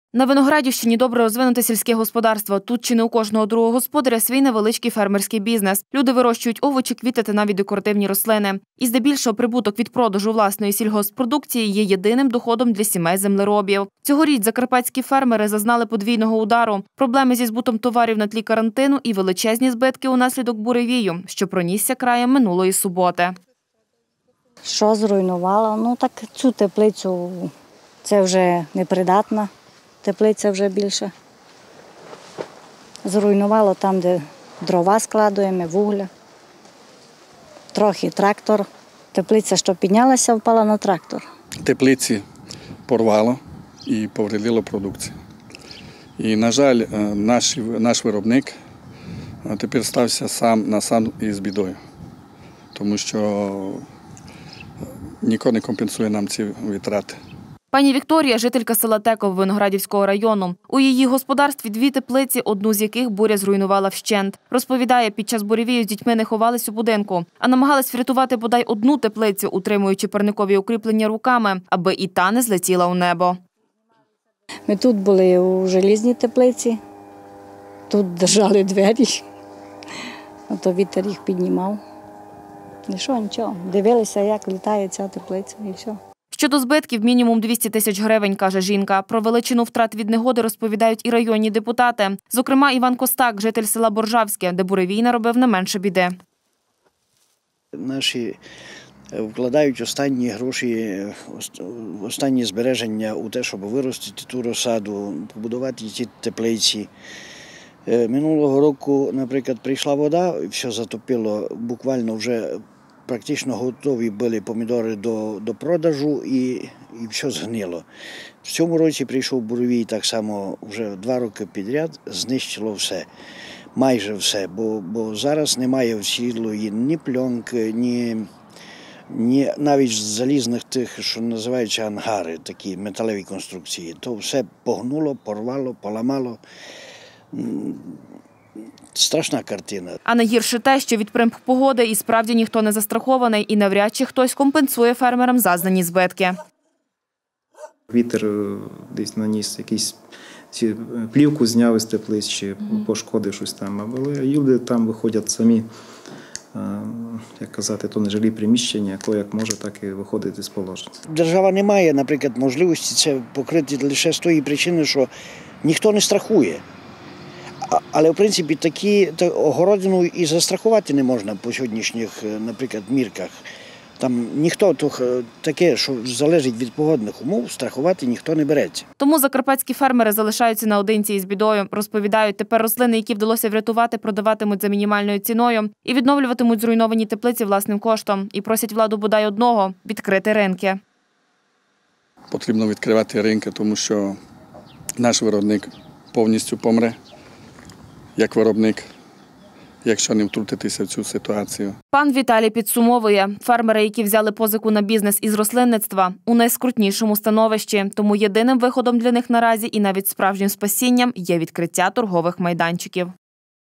Фермерські втрати
На Виноградівщині добре розвинуте сільське господарство, тут чи не у кожного другого господаря – свій невеличкий фермерський бізнес: люди вирощують овочі, квіти та навіть декоративні рослини. І, здебільшого,прибуток від продажу власної сільгосппродукції є єдиним доходом для сімей землеробів. Цьогоріч закарпатські фермери подвійного удару: проблеми зі збутом товарів на тлі карантину і величезні збитки унаслідок буревію, що пронісся краєм минулої суботи. Пані Вікторія - жителька с. Теково Виноградівського району. У її господарстві – 2 теплиці, одну з яких буря зруйнувала вщент. Розповідає, під час буревію з дітьми не ховались у будинку, а намагались врятувати бодай одну теплицю, утримуючи парникові укріплення руками, аби і та не злетіла у небо.Щодо збитків – мінімум 200 тисяч гривень, каже жінка. Про величину втрат від негоди розповідають і районні депутати. Зокрема, Іван Костак – житель села Боржавське, де буревій наробив не менше біди.А найгірше те, що від примх погоди і справді ніхто не застрахований, і навряд чи хтось компенсує фермерам зазнані збитки.Тому закарпатські фермери залишаються наодинці із бідою. Розповідають, тепер рослини, які вдалося врятувати продаватимуть за мінімальною ціною, і відновлюватимуть зруйновані теплиці власним коштом. І просять владу бодай одного - відкрити ринки.
 Пан Віталій додає, фермери, яківзяли позику на бізнес із рослинництва -  у найскрутнішому становищі. Тому єдиним виходом для них наразі і навіть справжнім спасінням є відкриття торгових майданчиків.